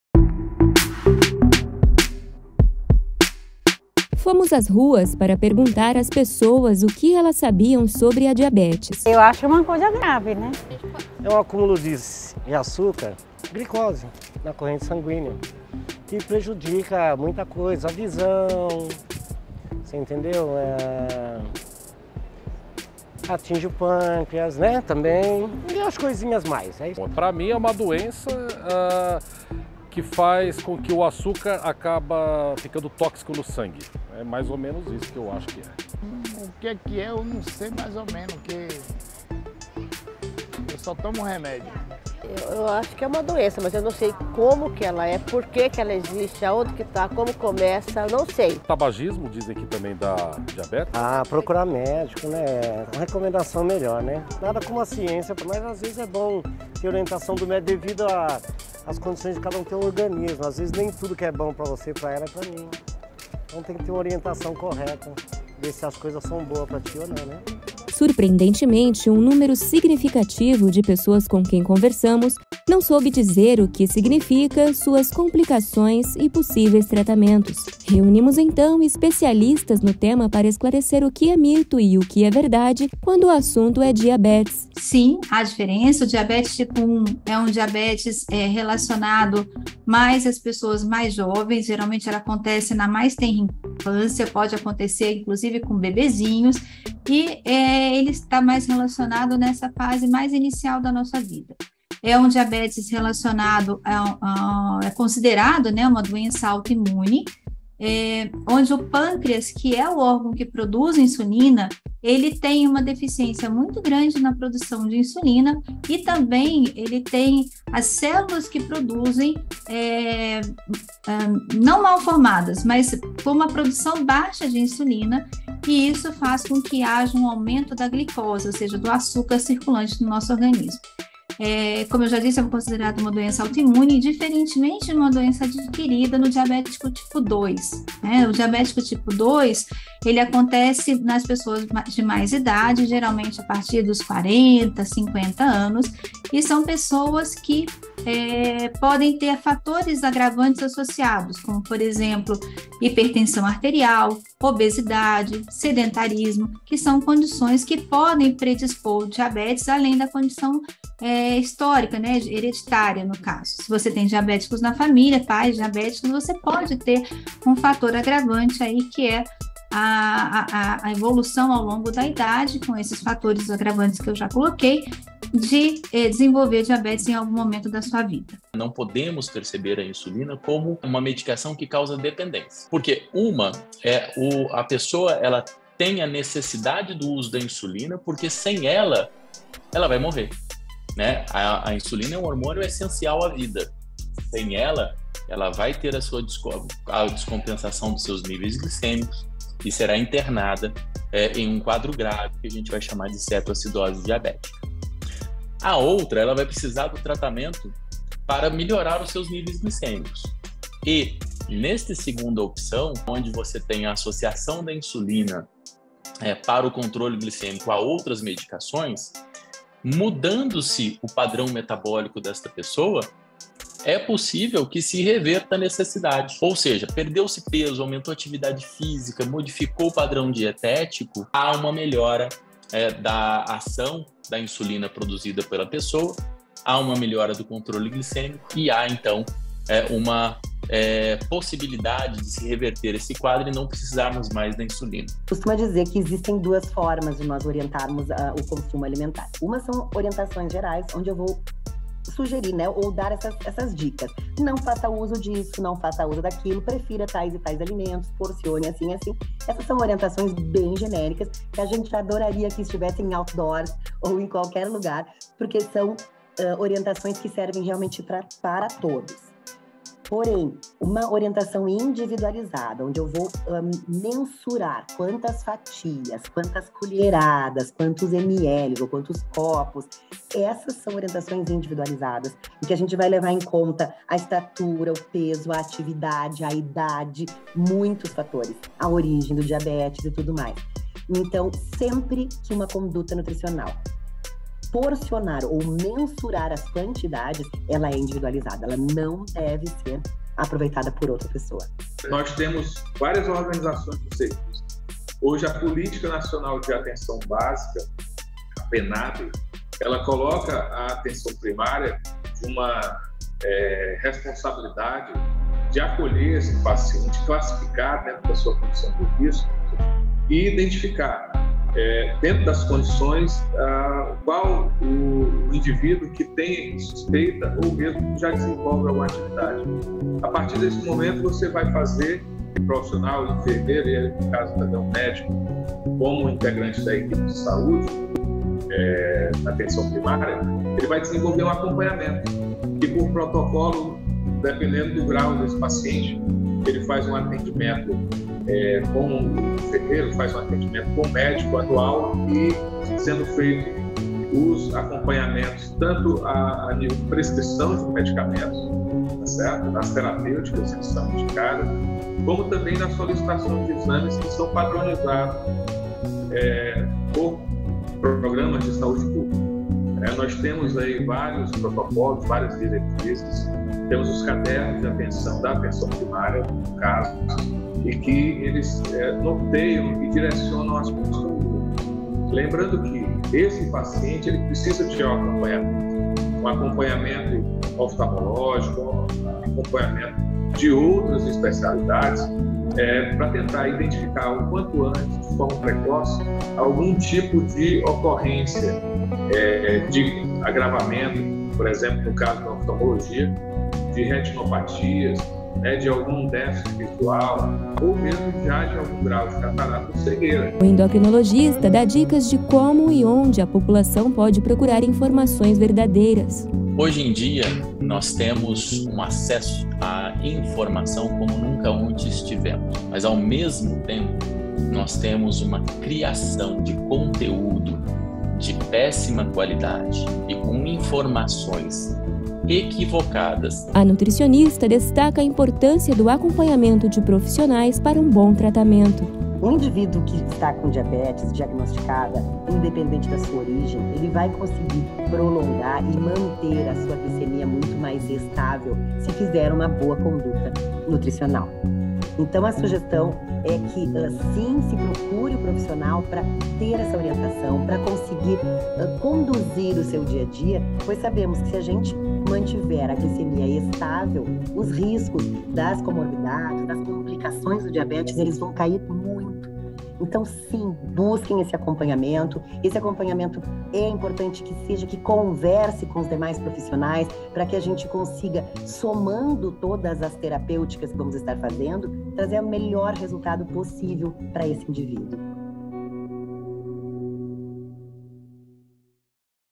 Mitos e verdades sobre diabetes
Reunimos especialistas de diferentes áreas, que compõe a equipe multidsciplinar no tratamentro de diabéticos, para esclarecer alguns mitos e verdades com relação a essa condição crônica de saúde.

O "24 Horas pelo Diabetes" é uma grande mobilização de concientização e combate à diabetes, que conta com a participação de entidades médicas de destaque em todo o país e especialistas para levar informação de qualidade sobre a doença.